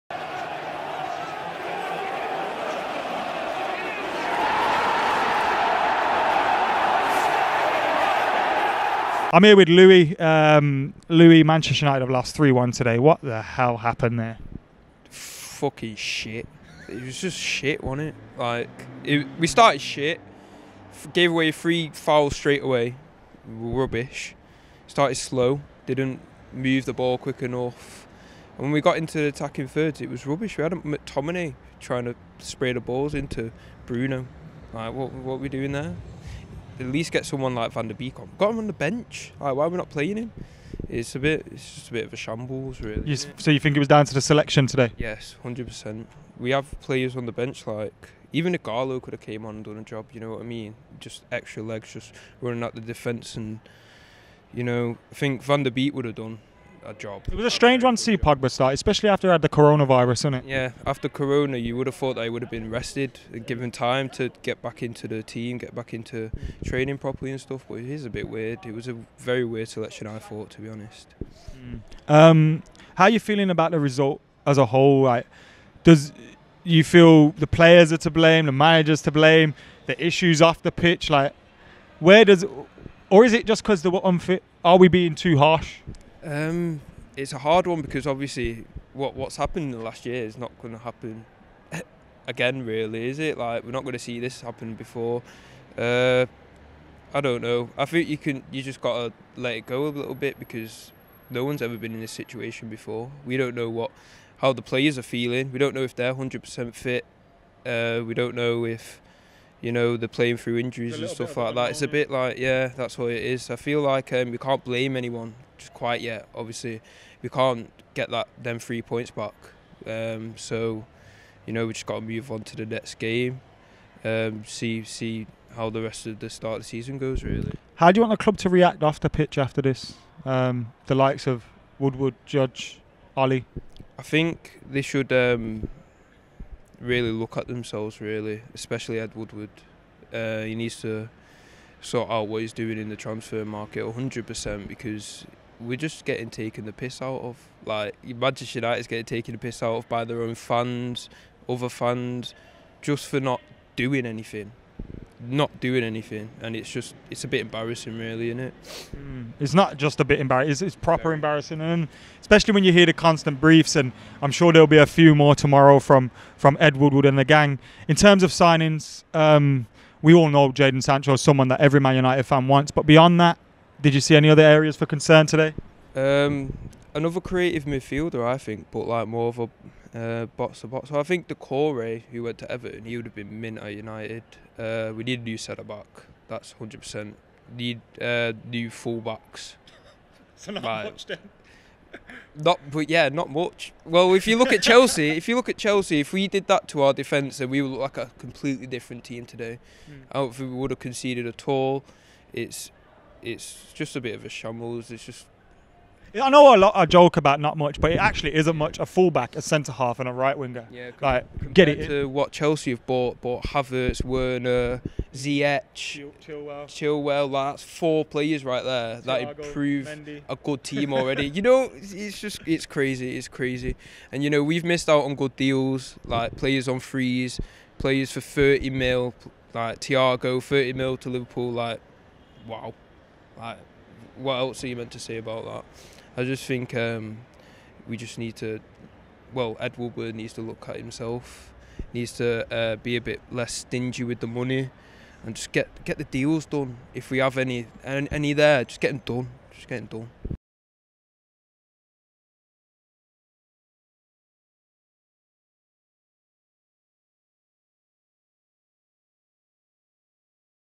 It Was An Absolute Shambles! | Louis' Fancam | Manchester United 1-3 Crystal Palace
Adam spoke to Louis from Stretford Paddock FC about tonights game and here's what he had to say!

Information on Training and Matches for Stretford Paddock FC can be found here; or the merchandise can be purchased from here;